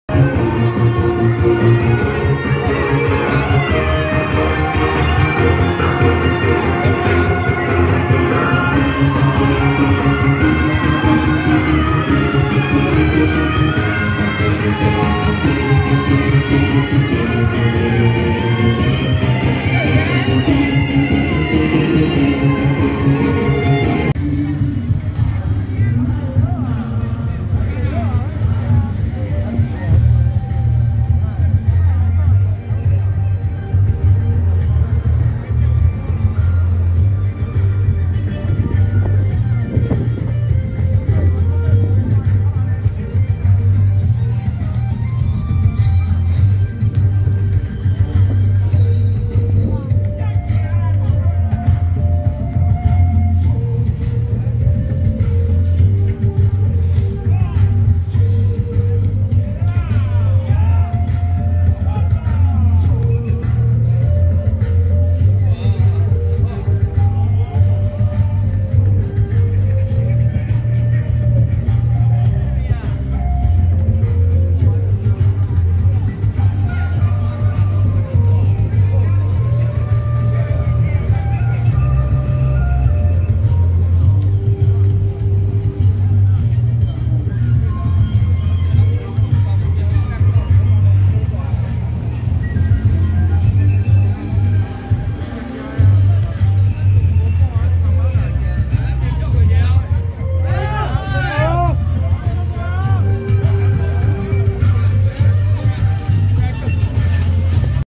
【北邑聖安堂-保安廣澤尊王 聖駕】~111三重武聯堂北極玄天上帝徒步南巡謁祖進香回駕
北邑聖安堂-保安廣澤尊王 聖駕